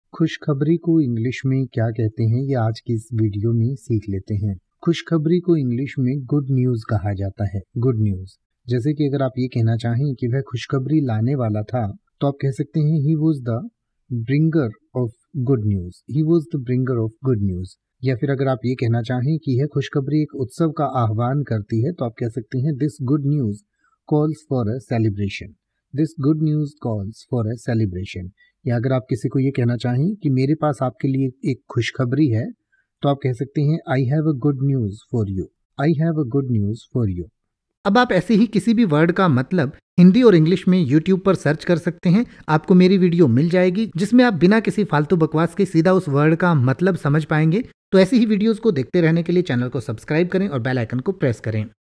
खुशखबरी को इंग्लिश में क्या कहते हैं | khushkhabri ko English mein kya kahate hain
इस वीडियो में आप खुशखबरी का हिंदी में मतलब समझेंगे और साथ में खुशखबरी का pronunciation भी सीखेंगें यानि की आप खुशखबरी के अर्थ के साथ साथ ये भी सीखेंगें की खुशखबरी को कैसे बोले या खुशखबरी को बोलने का सही तरीका क्या है |

तो बस एक मिनट में सीखिए खुशखबरी को|
Let’s learn Hindi Meaning of khushkhabri in detail.

खुशखबरी का अंग्रेजी में अर्थ होता है
Good News

He was the bringer of good news.
वह खुशखबरी लाने वाला था।

This good news calls for a celebration.
यह खुशखबरी एक उत्सव का आह्वान करती है।